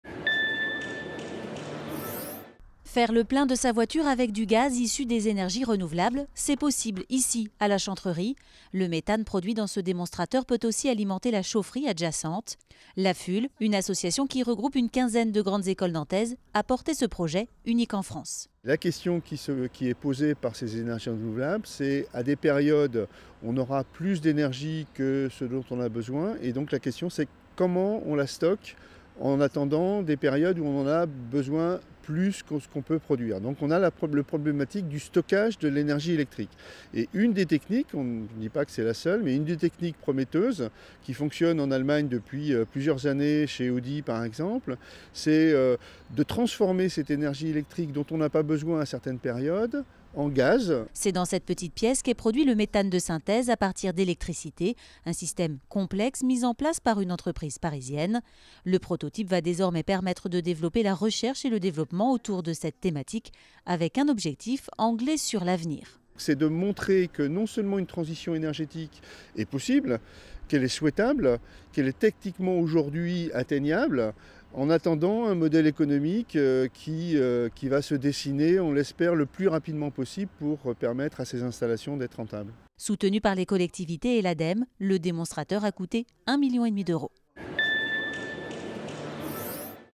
Interview de Bernard Lemoult à l'occasion de l'inauguration du démonstrateur MINERVE power-to-gas
Transition énergétique : Interview de Bernard Lemoult co-responsable développement durable et responsabilité sociétale IMT Atlantique et directeur de l'AFUL Chantrerie.
 Interview Télénantes du 8 février 2018 - version courte.